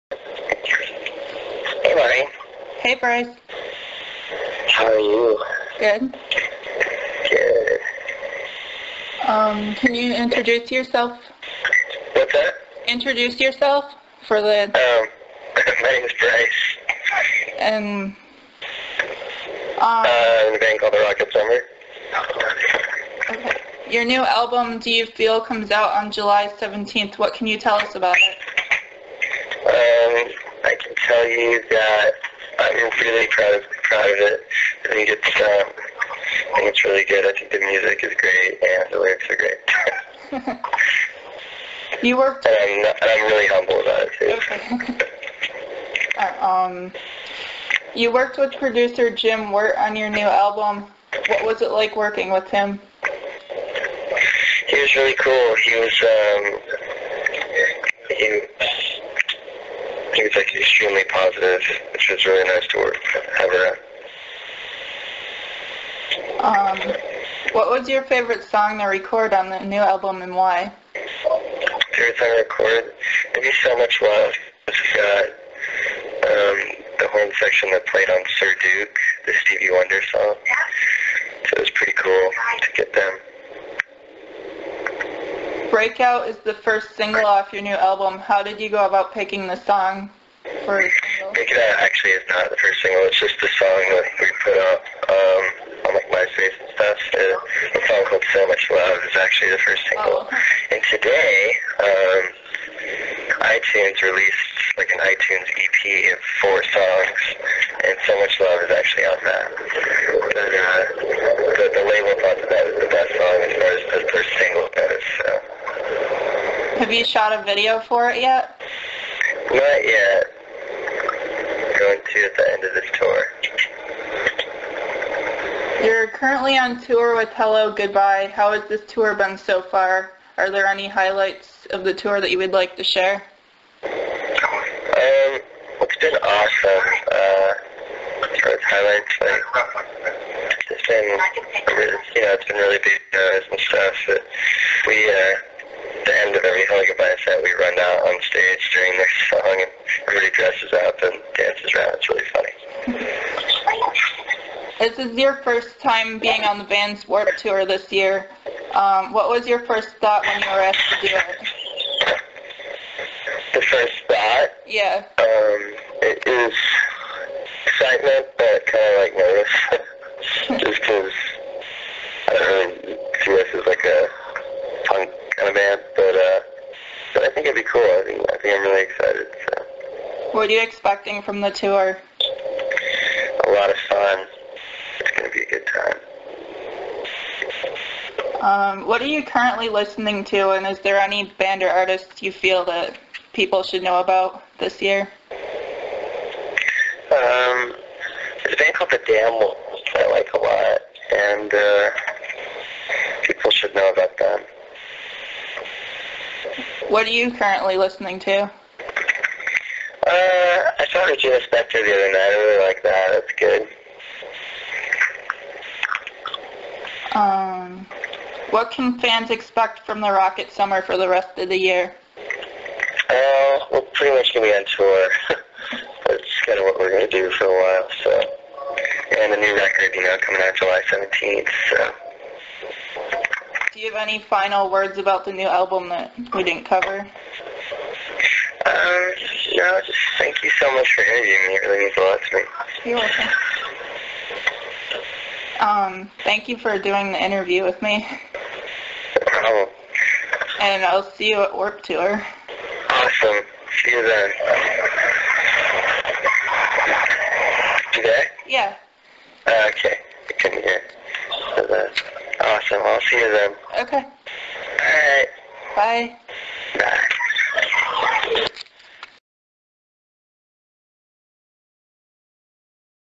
The Rocket Summer 2007
Burning Stars interviews Bryce Avary of The Rocket Summer via phone on May 22, 2007.